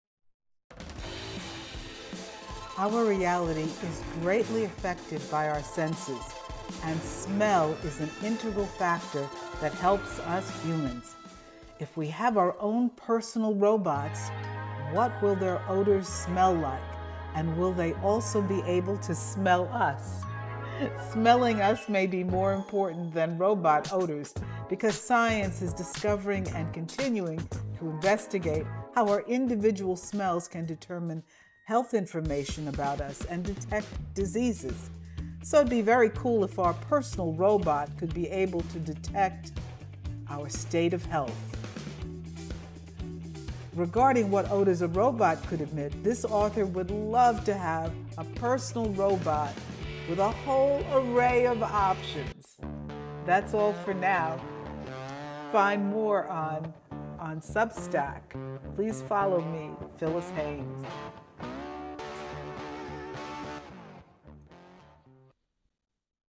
Robot Body Odor?
Will humanoid robots have body odor?